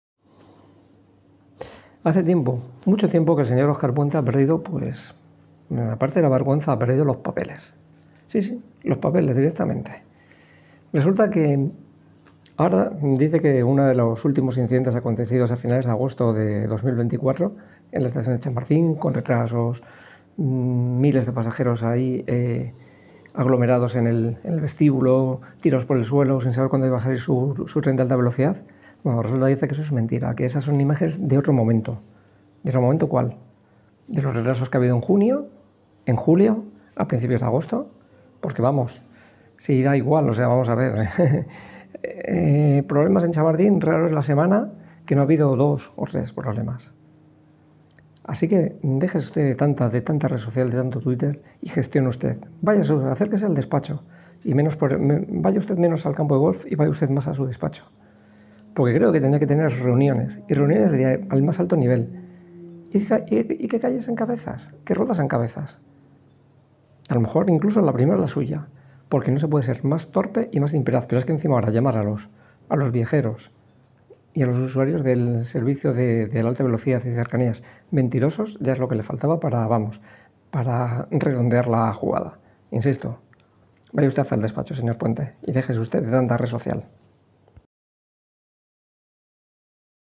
Óscar Puente (PSOE) vitupera a los viajeros de Renfe y se queda tan ancho
Lo que le faltaba al ministro de Transportes, no solo no asumir sus responsabilidades, sino además llamar mentirosos a los usuarios de Renfe.